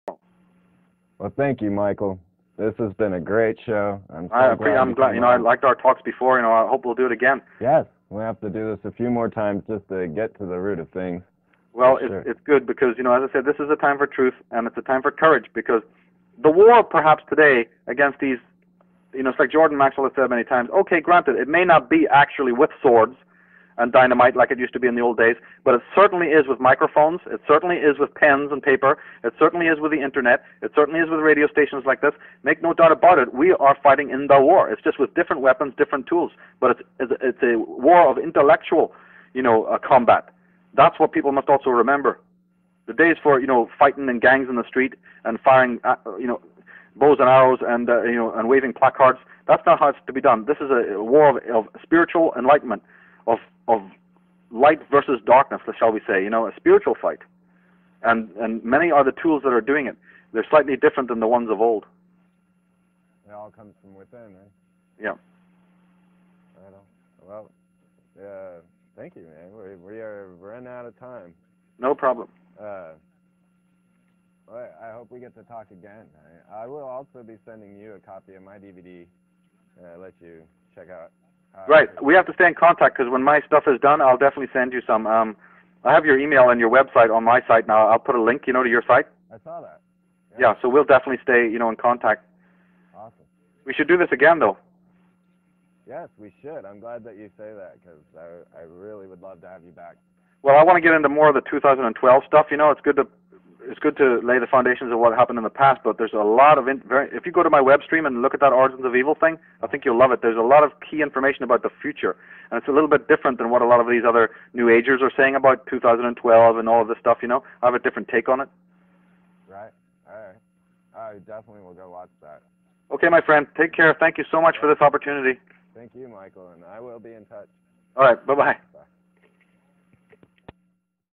Freeman With Michael Tsarion -Part 1 5/5 (Atlantis, Pre-History, Nephlim and Fallen Angels)
Lecturer, teacher and author Michael Tsarion, an expert on the occult, ancient societies and symbolism talks to Freeman of Freeman.TV about ancient earth history, Celtic history and how it holds many keys to understanding earths past. He also discusses occult symbolism, the origins of man, the ancient angelic wars and much more.